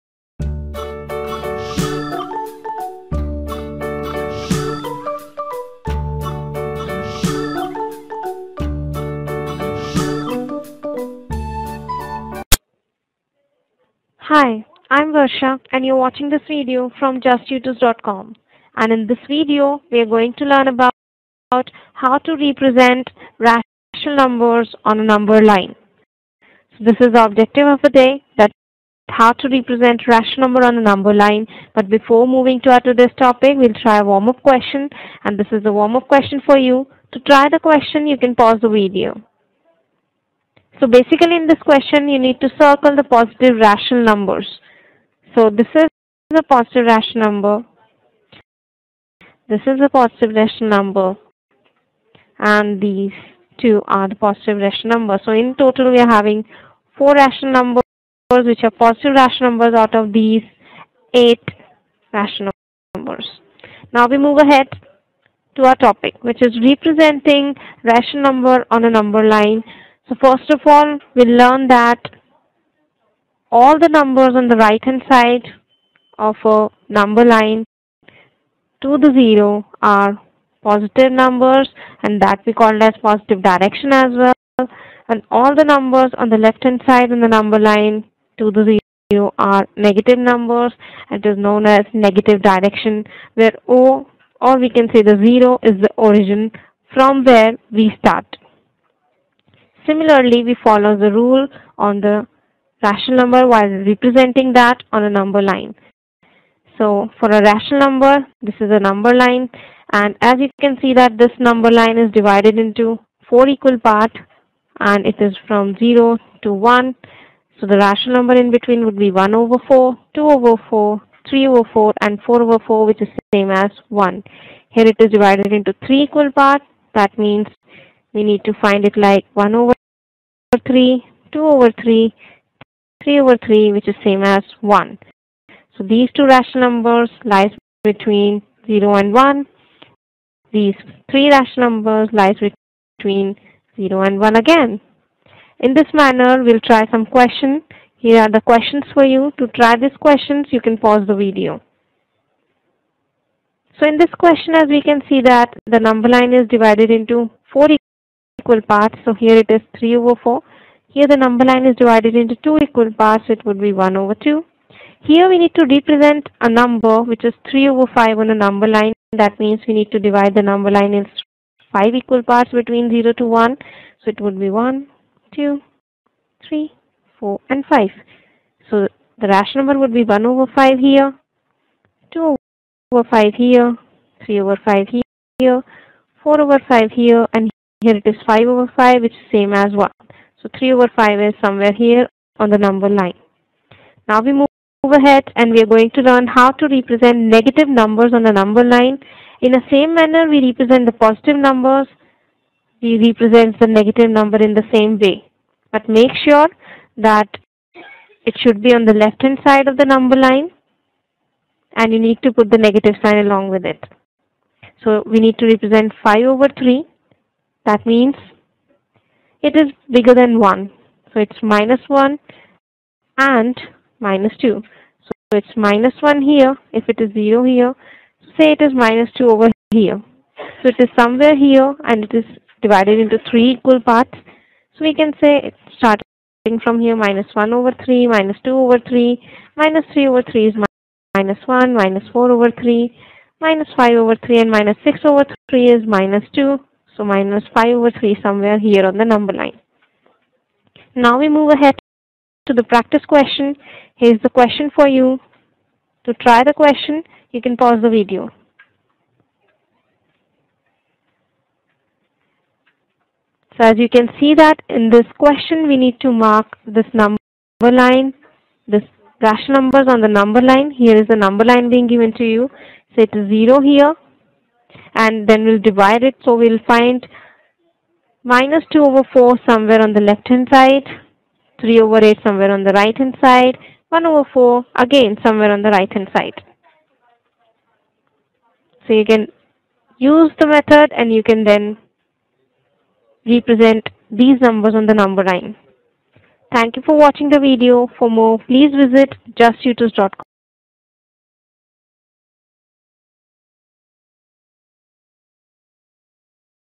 Rational Numbers: Representation of Rational Numbers on the Number Line
This video is all about the representation of rational numbers on number line. We are well aware how to represent integers on number line, In order to represent rational numbers, we may work out in same manner. Here we are going to introduce us with Rational Numbers. Rational Numbers are ratios of integers and we may get one for any pair of integers. Each fraction is a rational number but each rational number need not to be a fraction. Rational numbers and fractions have slightest difference,that is rational numbers can be negative but a fraction cannot.